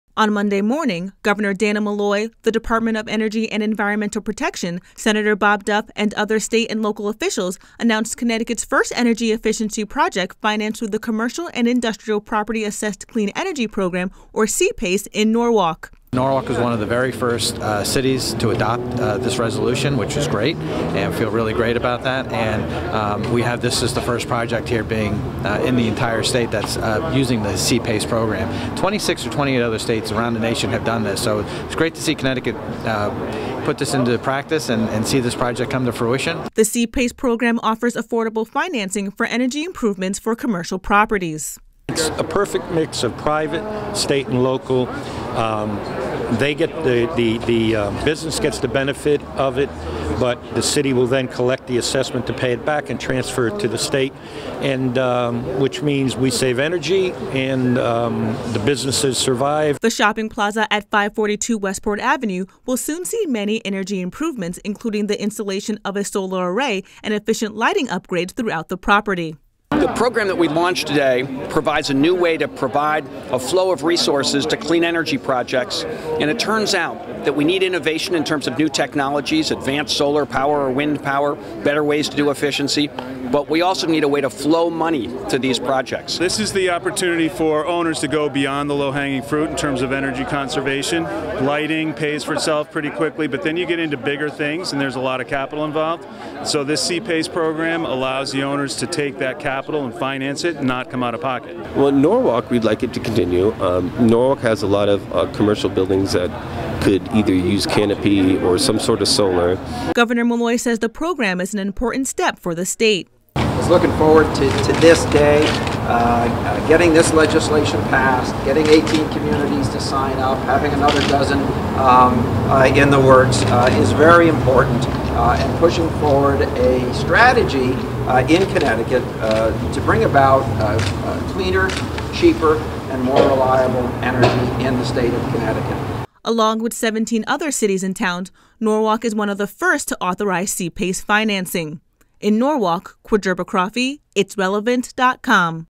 CT's First C-PACE Clean Energy Project in Norwalk
On Monday morning, Governor Dannel Malloy, the Department of Energy and Environmental Protection, Senator Bob Duff, and other state and local officials announced Connecticut's first energy efficiency project financed through the Commercial  Industrial Property Assessed Clean Energy programor C-PACE in Norwalk

The C-PACE program offers affordable financing for energy improvements for commercial properties.
The shopping plaza at 542 Westport Avenue will soon see many energy improvements including the installation of a solar array and efficient lighting upgrades throughout the property.
Governor Malloy says the program is an important step for the State.
Along with 17 other cities and towns, Norwalk is one of the first to authorize C-PACE financing